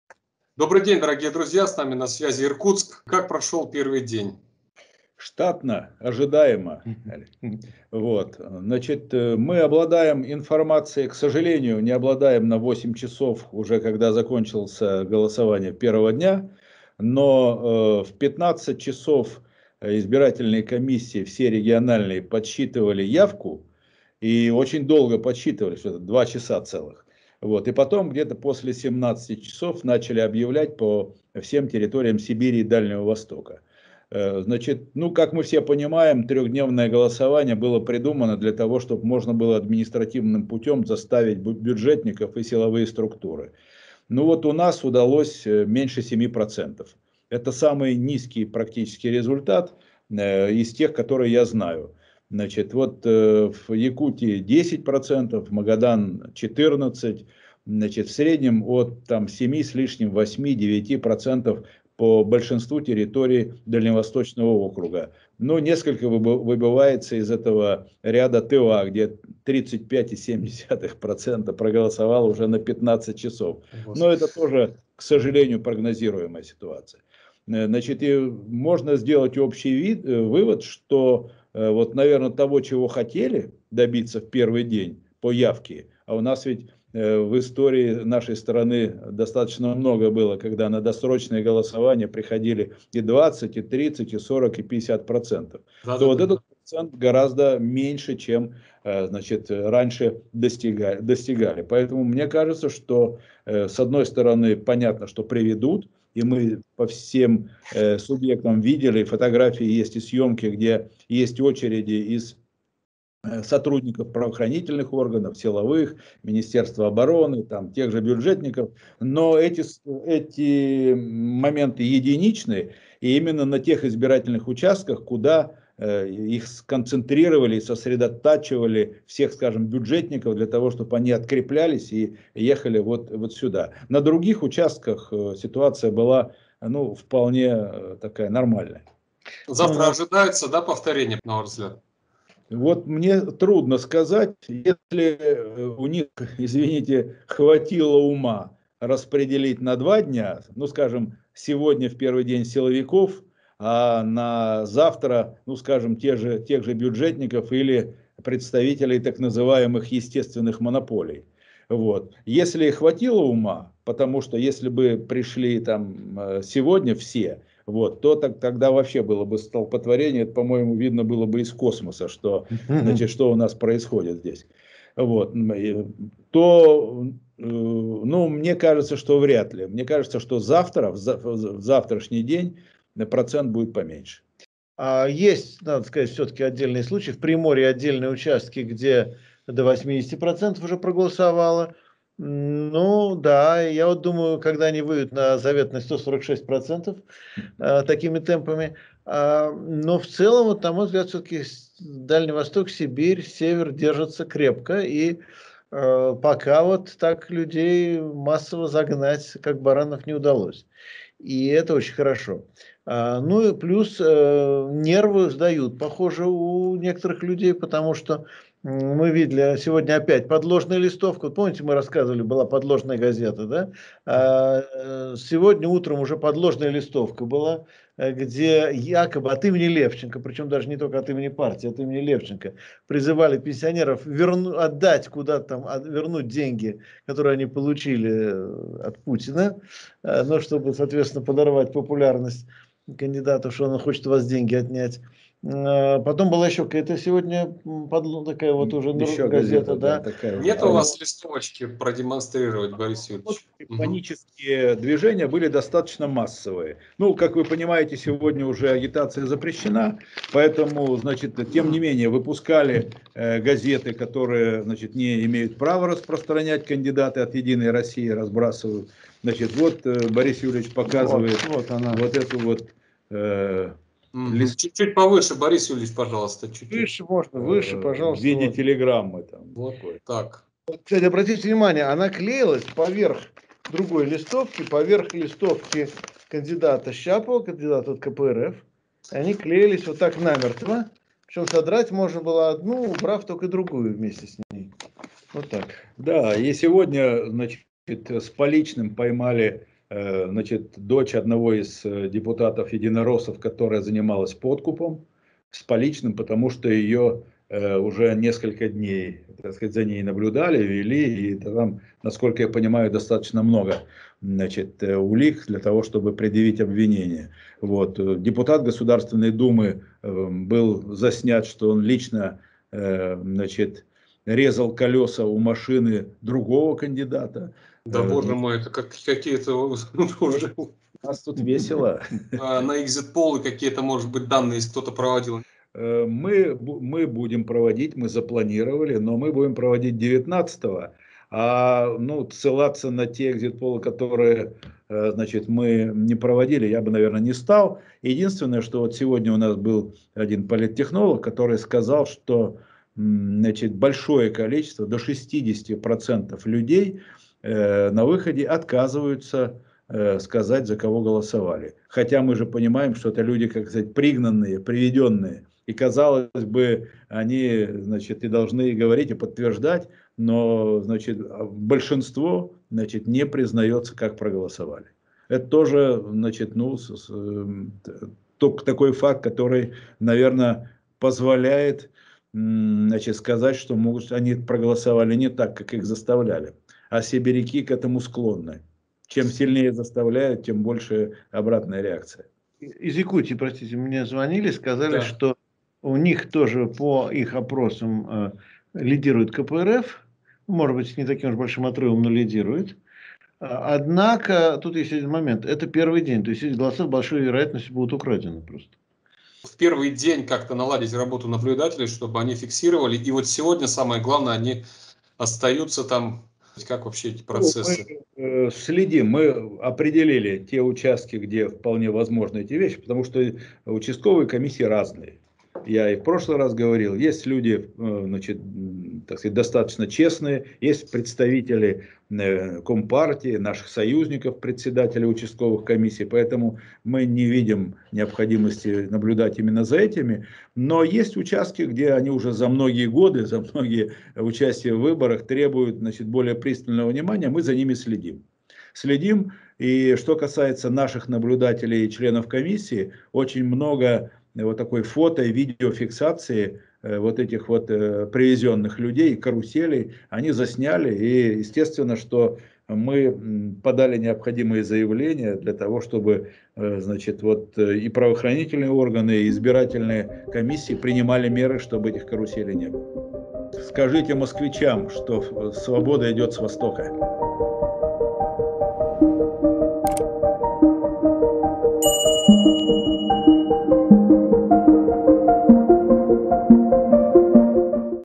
Левченко и Кагарлицкий о первом дне голосования
Олег Калачев связался с Иркутском, где сейчас находятся Сергей Левченко и Борис Кагарлицкий, и расспросил об итогах первого дня голосования.